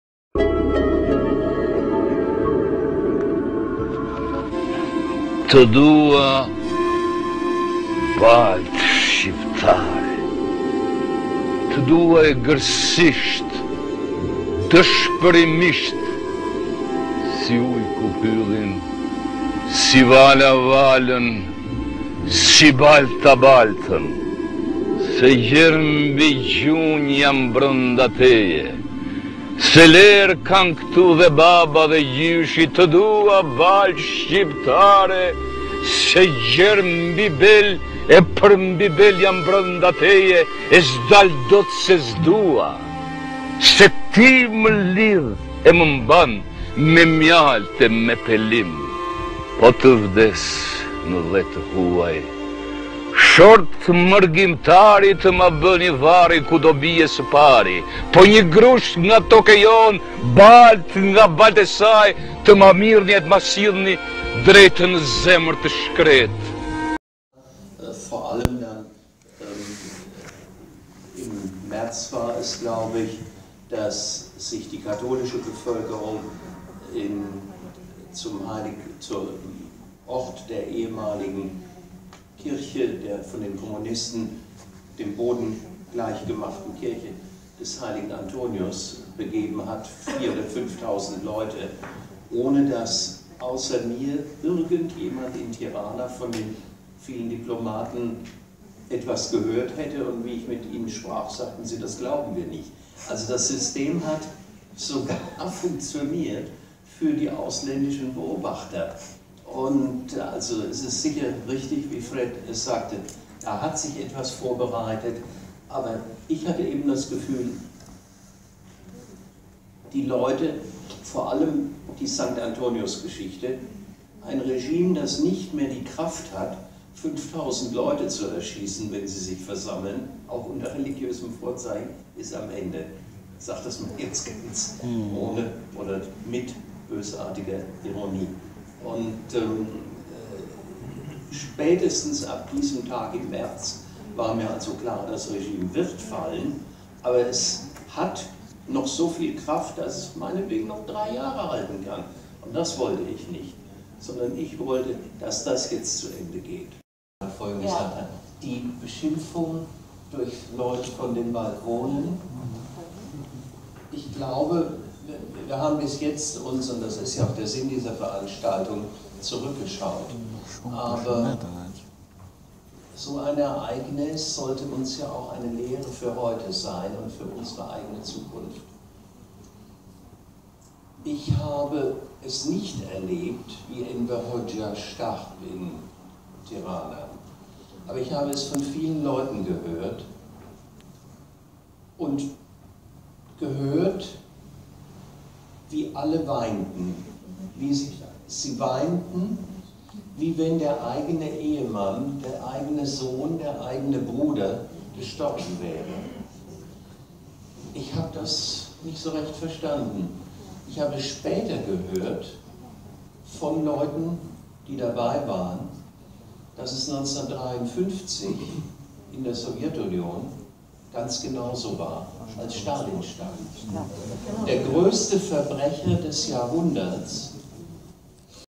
Me diplomatin Werner Daum në 25 - vjetorin e ngjarjeve të ambasadave.
diplomat, ish-ambasadori gjerman në Tiranë, Werner Daum, njeriu që u hapi derën dhe i ndihmoi shqiptarët e futur në ambasadën gjermane në korrik 1990, kujton rolin e ambasadave perëndimore në këtë ngjarje, sidomos atë Gjermane. " Isha i bindur se ky regjim që nuk mban dot mijëra vetë të hyjnë në ambasada, nuk e ka të gjatë.”